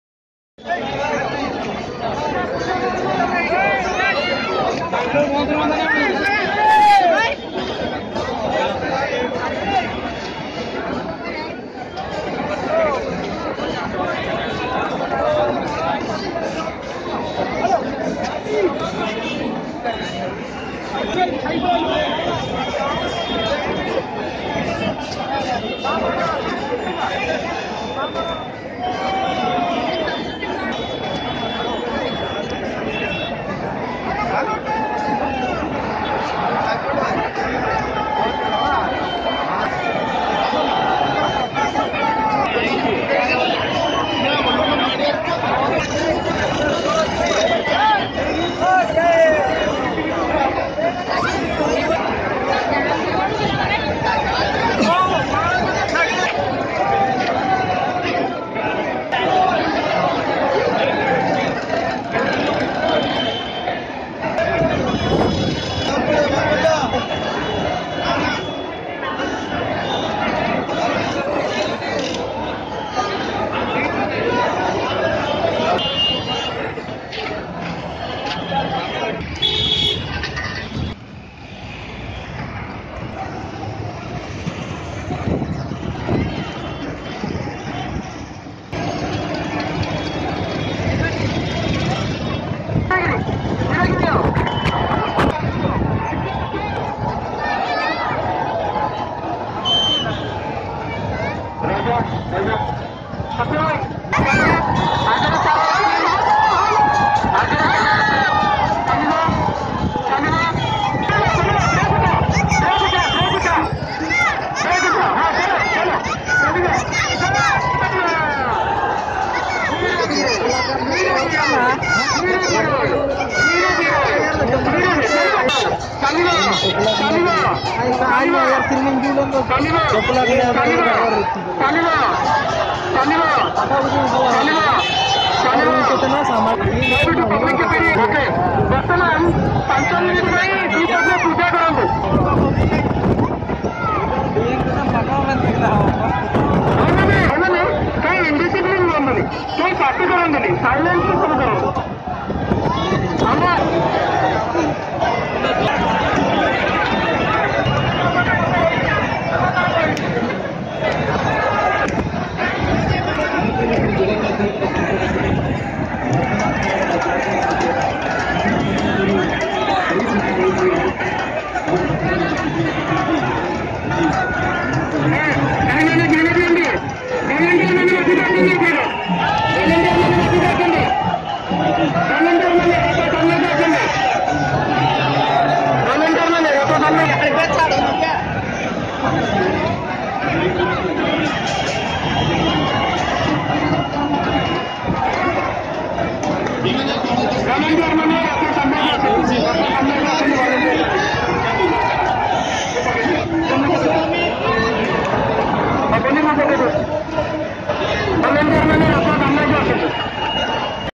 Bangalore Ratha Yatra 2017: Cart Festival at Agara, Bengaluru
World famous cart festival at Bangalore, Agara, Jagannath Temple. And it looks like second Puri Ratha Yatra. Jagannath Dham Puri & Agara, Bangalore Looks like same.